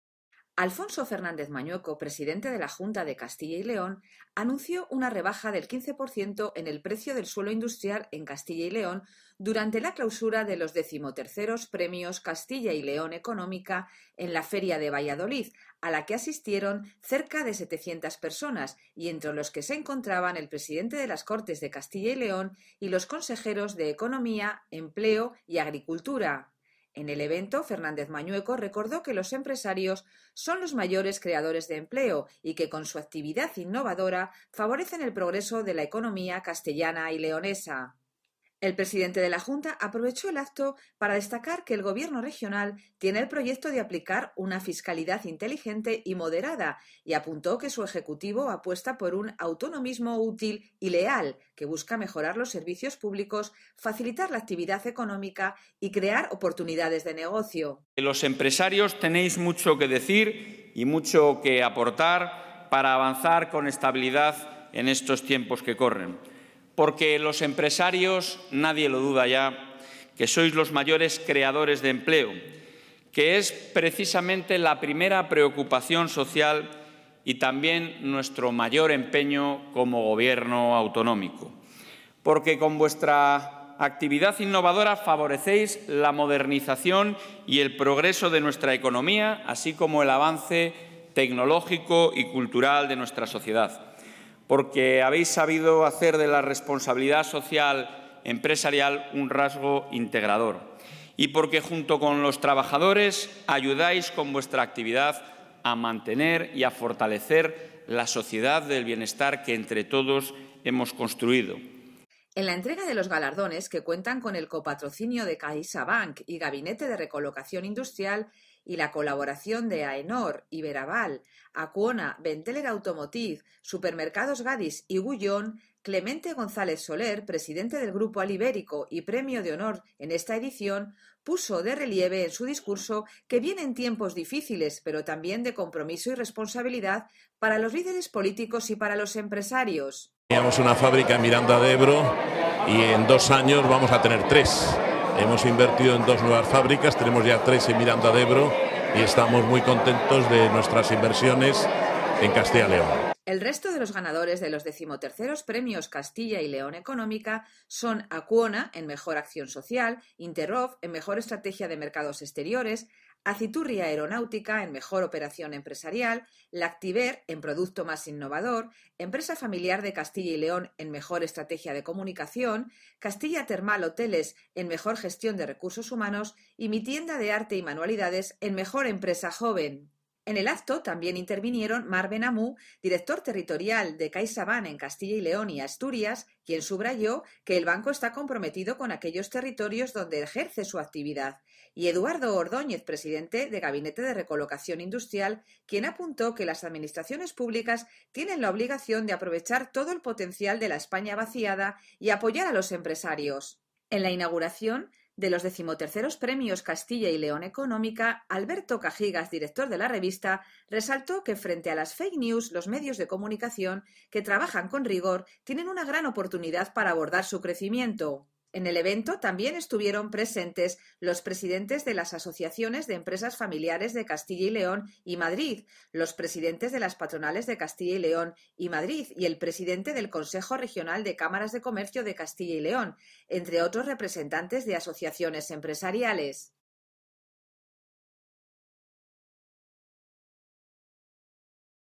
Gala de Entrega de los XIII Premios Castilla y León Económica
Alfonso Fernández Mañueco, presidente de la Junta de Castilla y Léon, anunció una rebaja del 15% en el precio del suelo industrial en Castilla y León durante la clausura de los XIII Premios Castilla y León Económica en la Feria de Valladolid, a la que asistieron cerca de 700 empresario y directivos y en tre los que se encontraban el presidente de las Cortes de Castilla y León y los consejeros de Economía, Empleo y Agricultura.